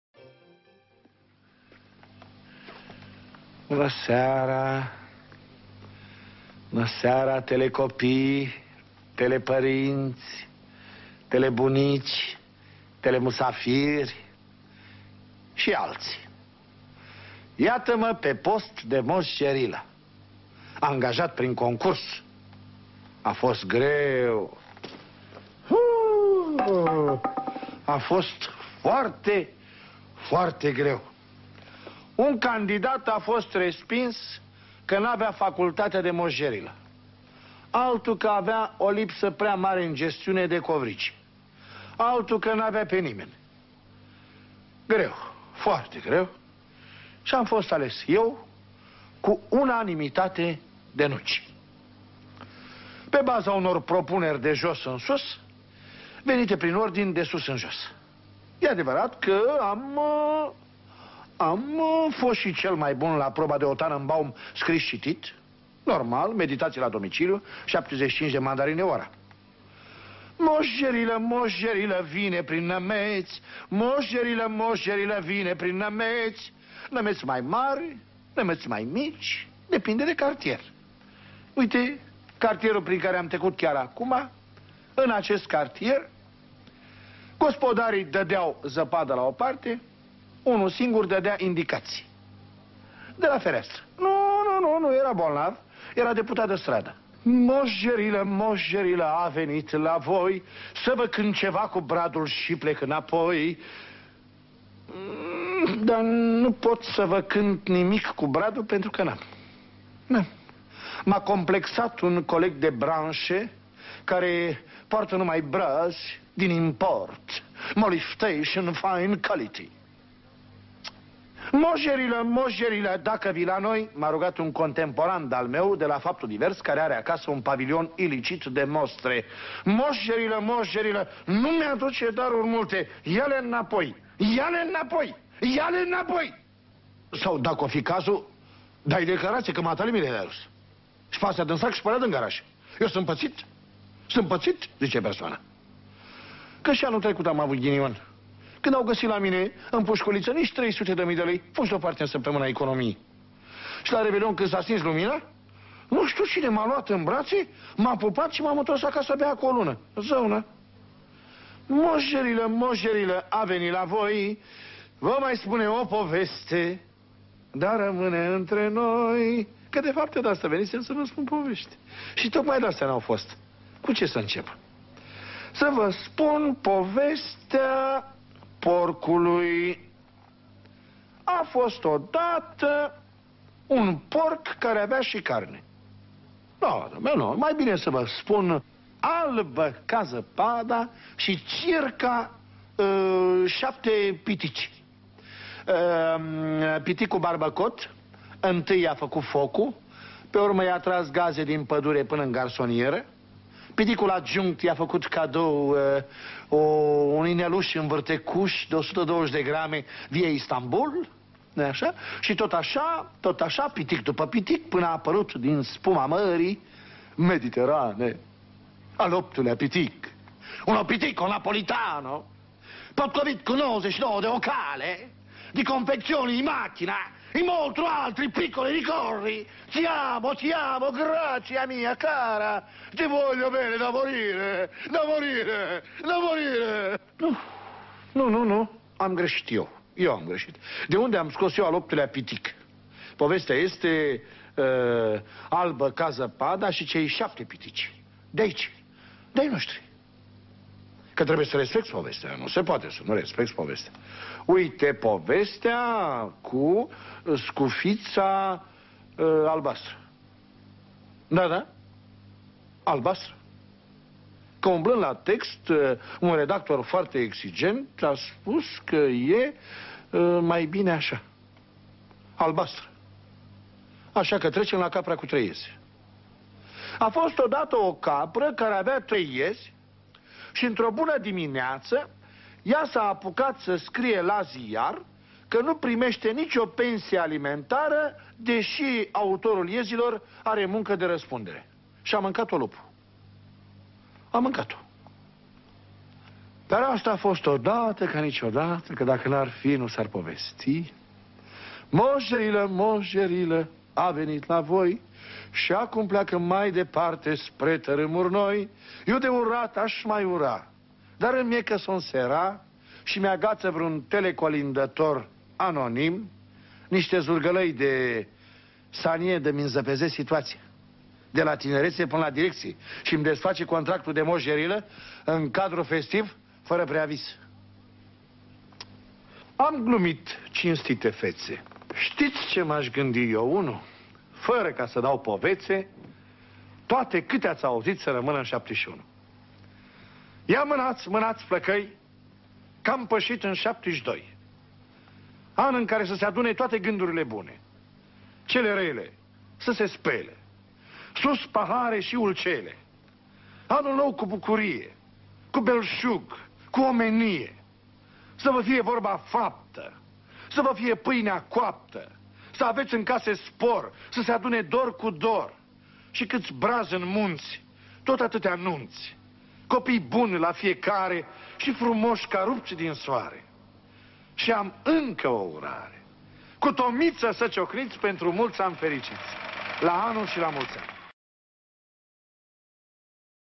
Mos Gerila, Mos Gerila - Toma Caragiu
Toma Caragiu povesteste cum a ajuns sa fie Mos Gerila. Si n-a fost usor deloc: un candidat a fost respins ca n-avea Facultatea de Mos Gerila, altul avea o lipsa prea mare in gestiune de covrigi.